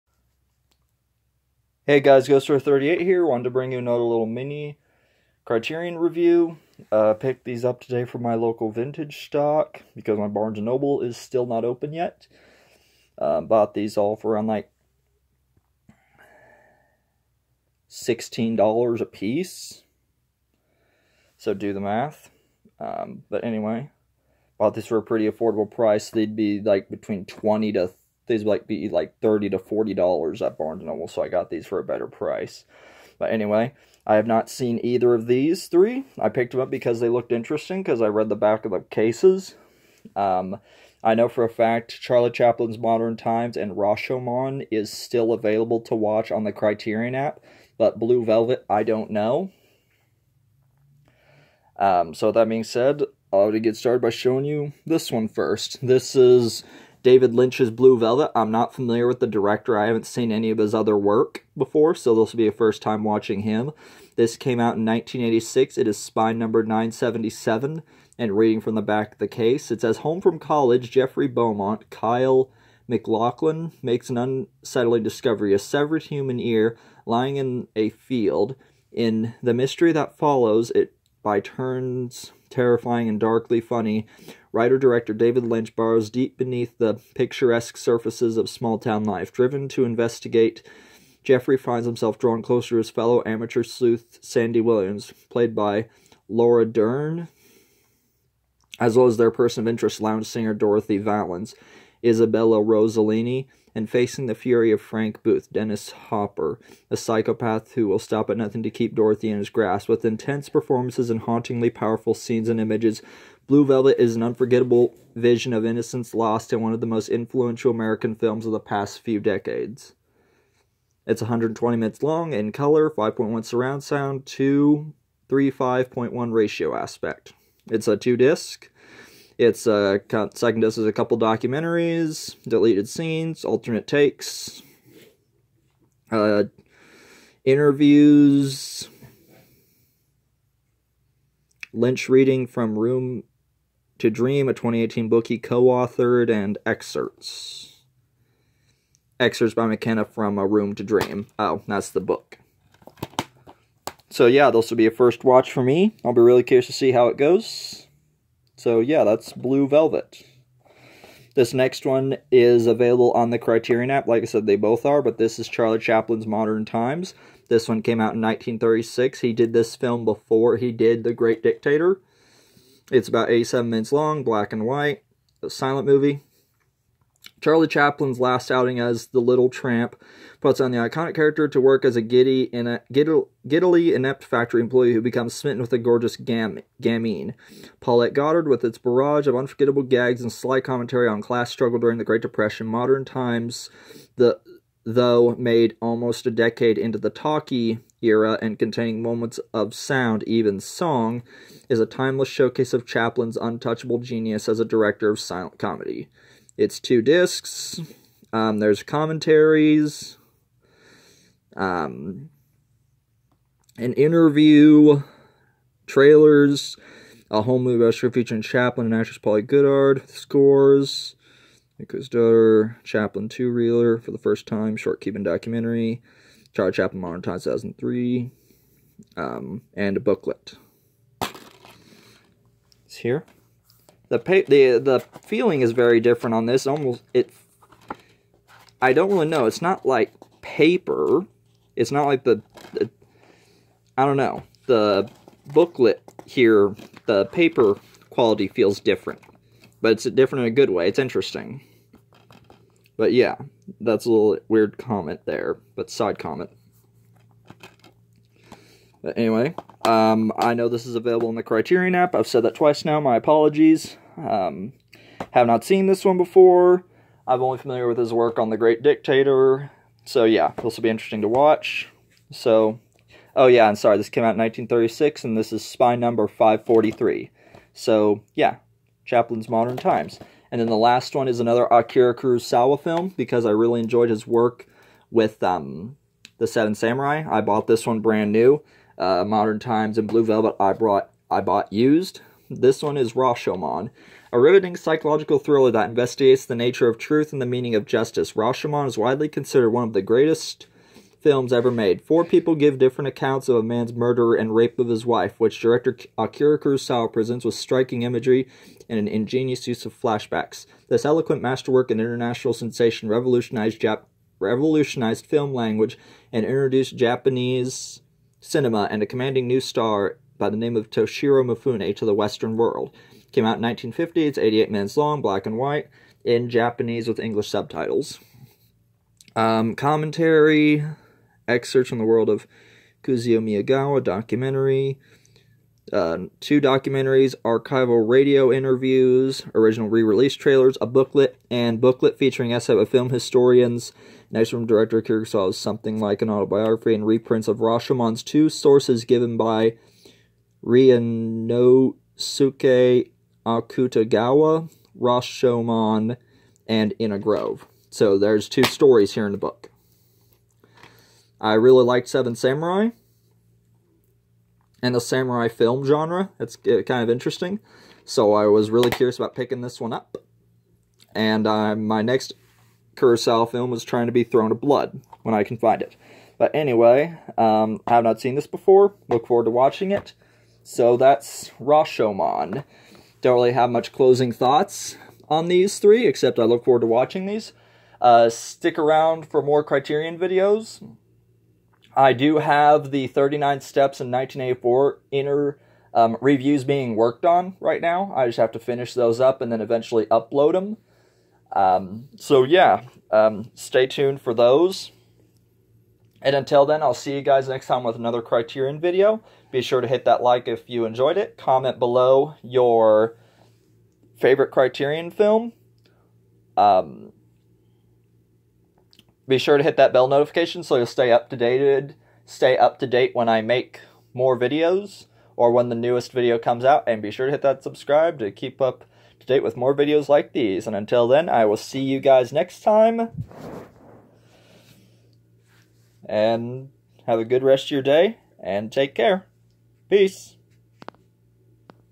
Criterion Collection Mini Review
Picked these up from my local vintage stock, and wanted to show what i picked up. Currently working on some other movie reviews as well. Ill be doing a video in the next few weeks if not month, and show my entire Criterion Collection i have thus far.